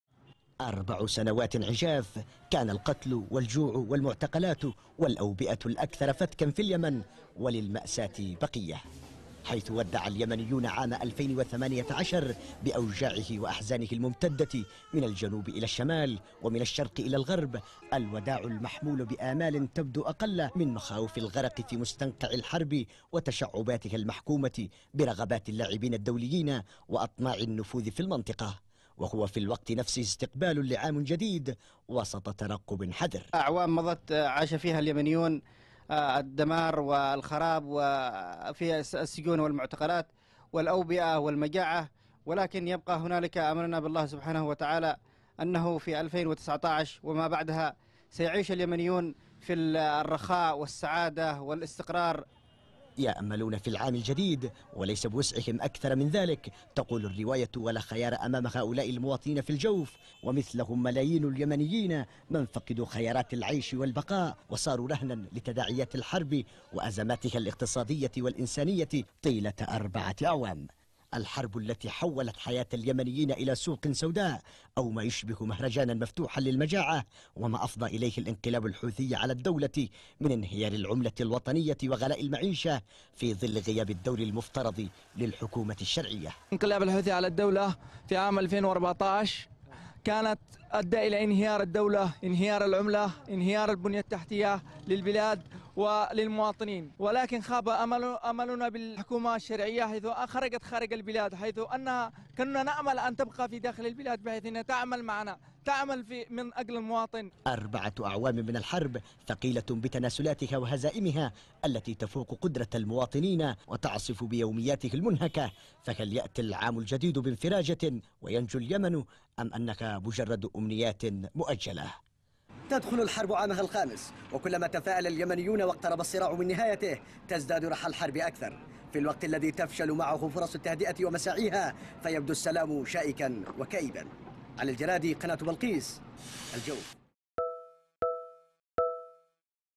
الجوف..أربعة أعوام من التدهور وسط تطلعات المواطنين لحياة أفضل خلال العام الجديد | تقرير: علي الجرادي
أربعة أعوام مضت ثقيلة بأوجاعها وتداعياتها الكارثية على حياة اليمنيين، كان القتل والجوع والمعتقلات والأوبئة الفتاكة أبرز مظاهرها، في ظل مساعي النفوذ والأطماع الإقليمية والدولية، التي تسببت بها حرب المليشيات الحوثية وانقلابها على الدولة، الحرب التي حولت حياة اليمنيين إلى سوق سوداء.. فهل يأتي العام الجديد بانفراجة وينجو اليمنيون، أم انها كسابقاتها  مجرد أحلام مؤجلة.